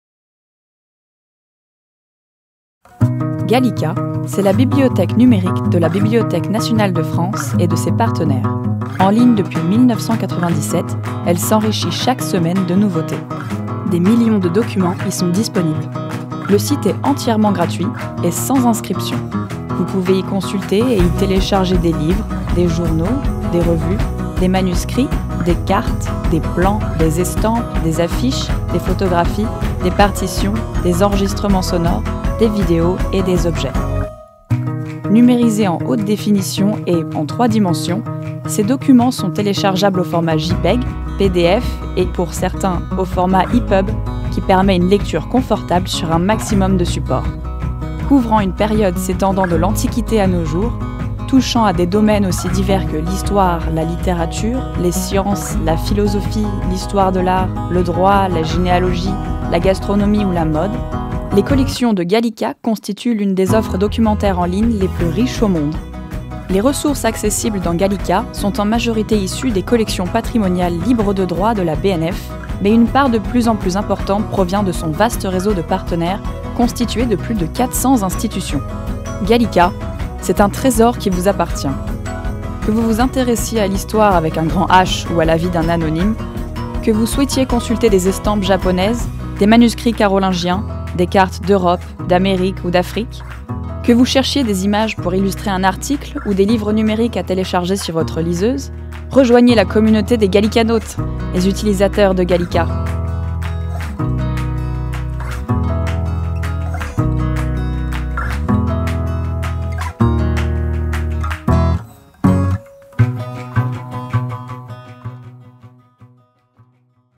Gallica, qu'est-ce que c'est ?
Gallica est la bibliothèque numérique de la Bibliothèque nationale de France (BnF) et de ses partenaires. Depuis 1997, elle offre un accès libre et gratuit à plusieurs millions de documents numérisés (livres, journaux, images…) de toutes époques. Sa collection s’enrichit, chaque année, de la coopération de centaines d’institutions avec la BnF.

Réalisation : Vagabondir
Voix-off : Mathilde Garrett